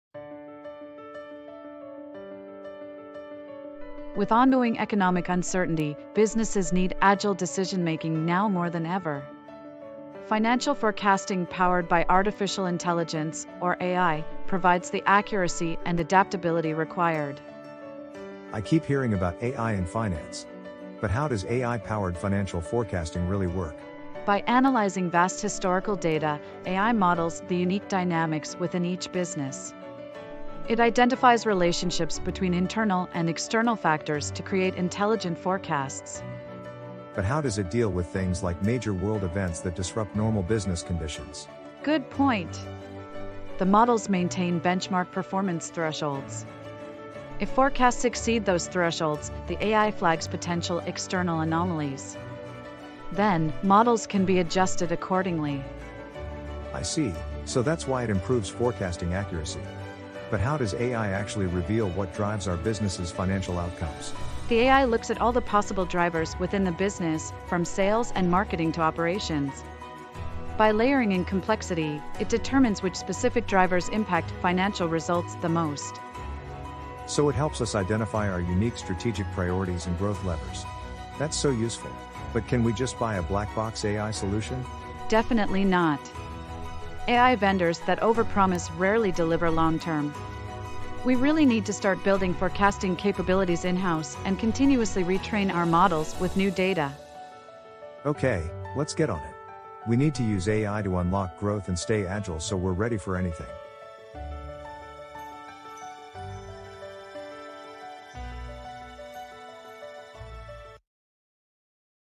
Why You Should Be Using AI in Financial Forecasting
Now is the time to be using AI-powered forecasting models to unlock growth and agility for your business. This quick video provides an overview of how intelligent forecasting with AI works to drive faster, data-driven decisions and  to give your business a competitive edge. Whether you're a business leader or an analytics professional, find out how you can incorporate AI to take the guesswork out of financial planning and strategic decision-making.